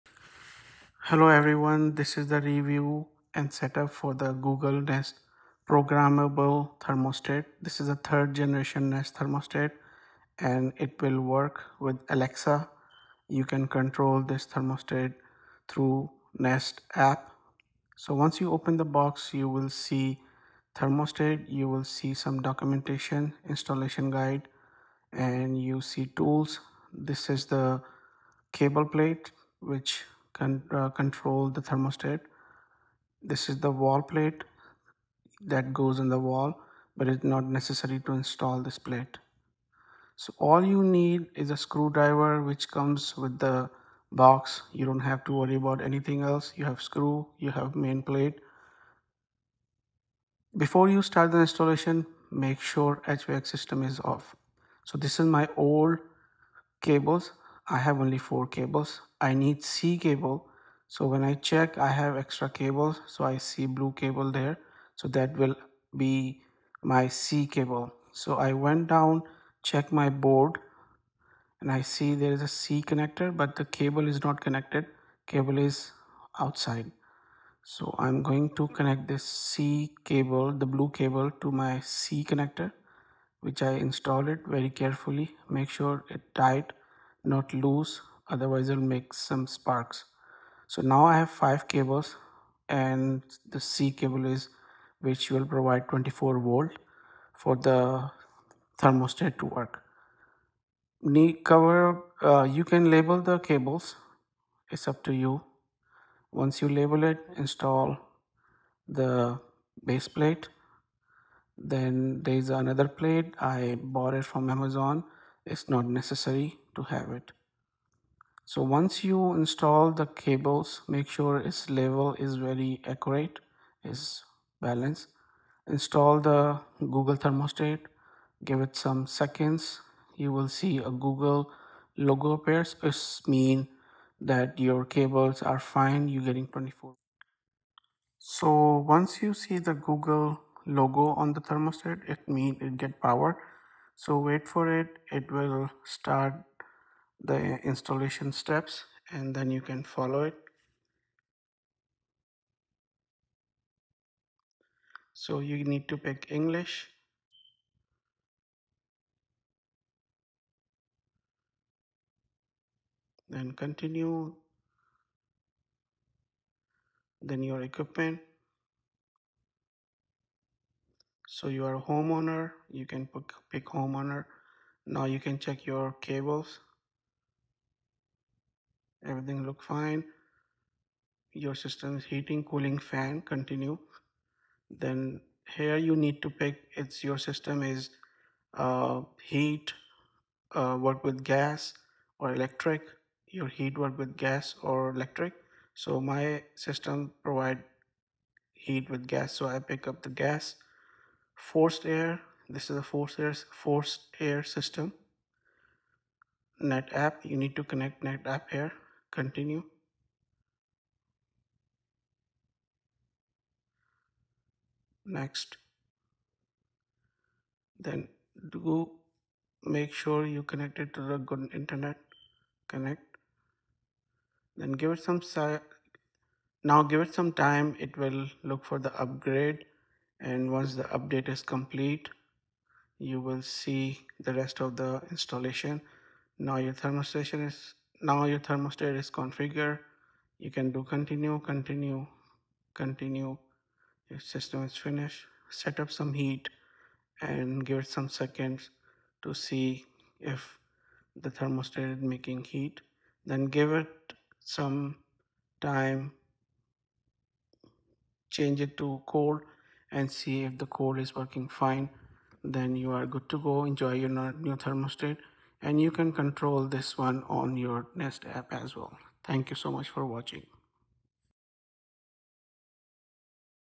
How to Replace HVAC Thermostat (How to Install Nest Thermostat) New 2023
How to replace an HVAC thermostat and install a Nest thermostat. It was time for me to replace my thermostat so I filmed the whole process of me installing a new Nest thermostat and tried to answer as many frequently asked thermostat installation questions as I could in the process. Hope the video helpful! Its DYI job, but careful doing anything with HVAC

Timecodes
0:10 – Intro
0:49 – Installation Begin
1:08 – Connecting C cable
2:06 – Setup Nest Thermostat